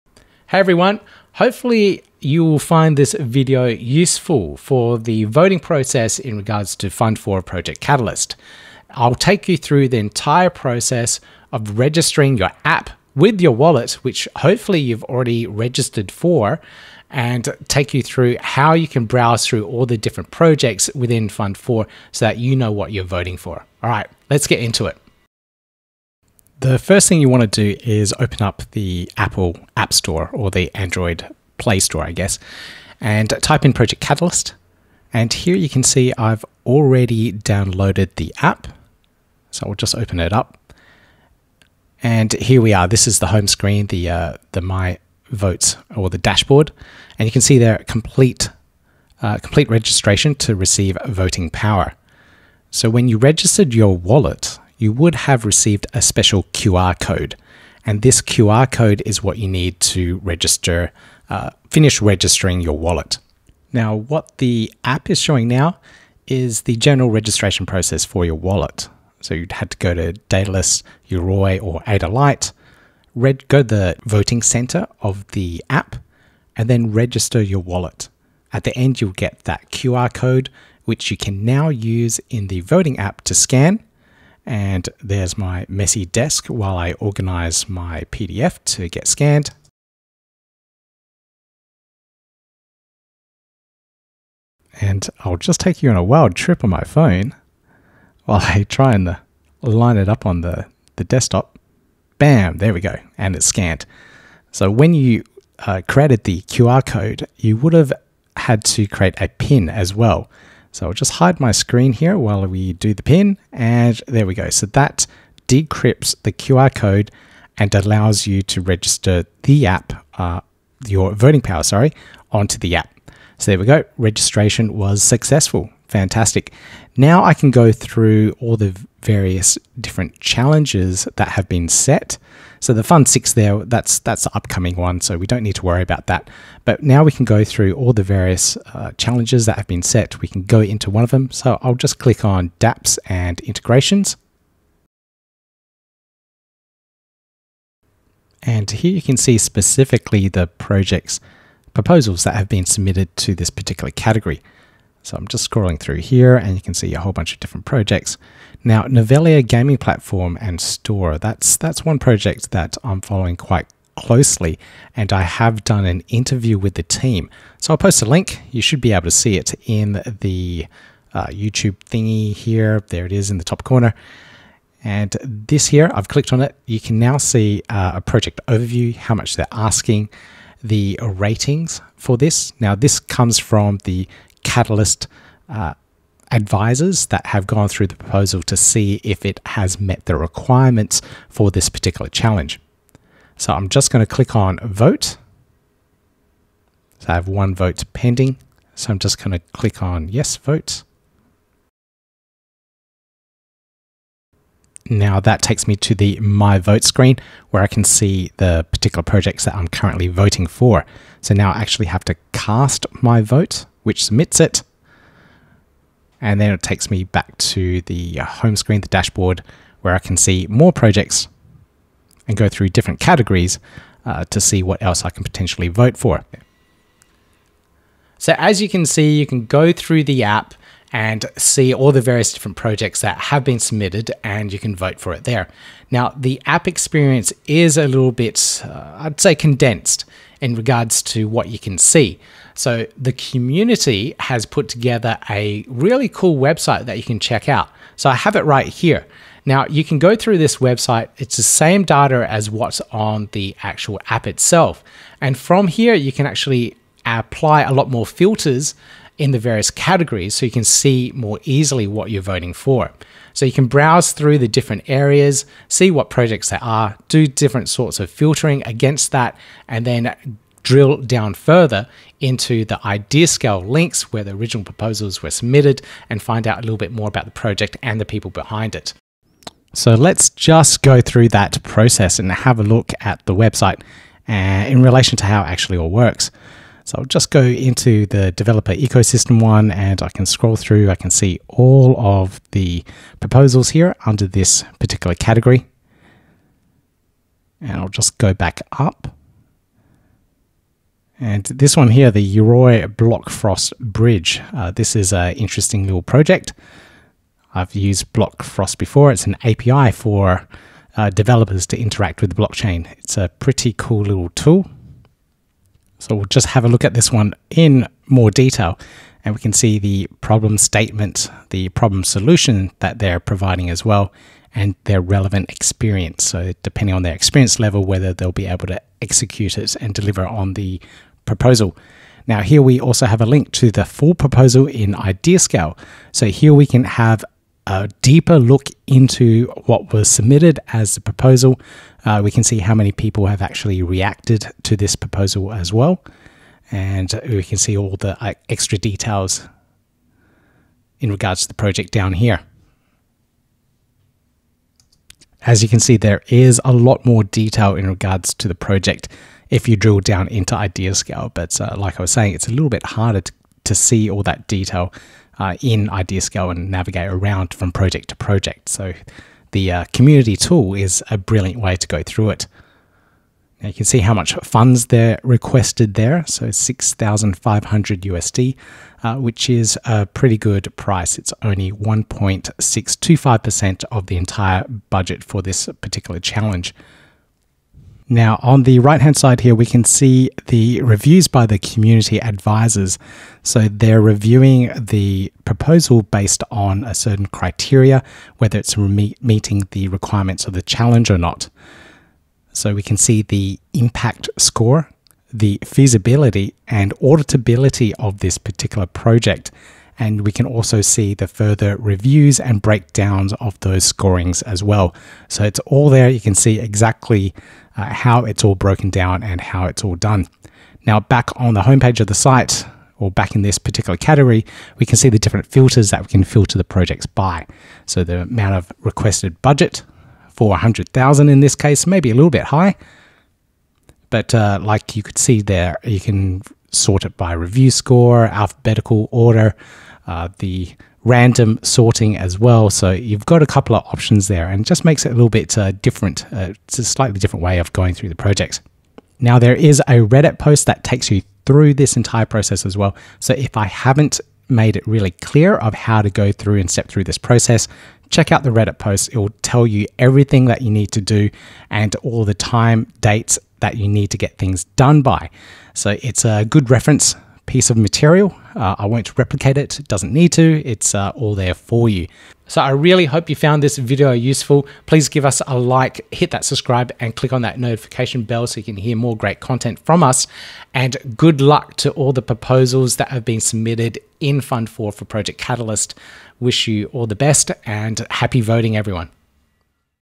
How to Vote in Project Catalyst Cardano
In this video I'll be taking you through the process of completing your registration for voting on the iOS version of the Project Catalyst app.

We'll download the app, complete the QR registration that is required to link your wallet with your app, and then show you how to navigate through all of the proposals that have been submitted to IdeaScale by using the app and the community built website called the Cardano Catalyst Voter Tool.

Good luck to all those that have submitted proposals and happy voting to everyone.

0:26 - Download the App
0:40 - Complete Your  Registration
1:18 - Use your registration QR code
2:06 - Navigating the projects
3:47 - Alternative view using Voter Tool
5:00 - What all the ratings mean
7:00 - Different ways to view proposals
7:28 - Full guide on Reddit
7:52 - Wrap up

Support the podcast and delegate to our stake pool, Ticker ADAOZ